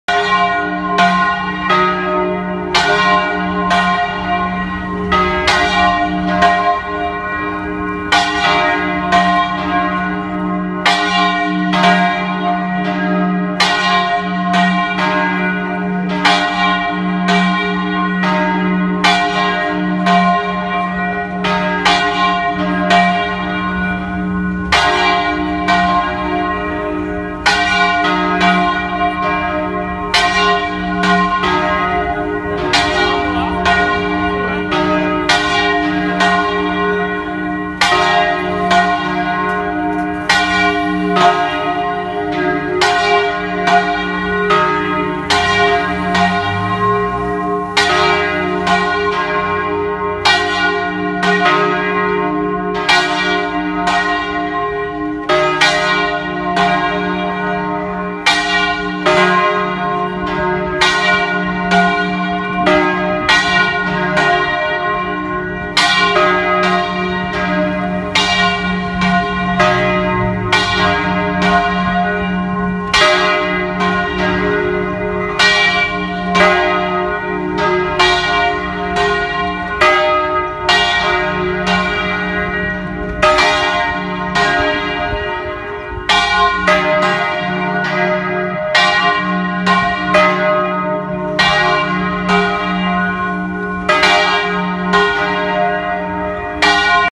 Sinos do São Francisco, São João Del Rei MG
Sinos do São Francisco de Assis tocando na festa do encontro
DATA: 31/03/2019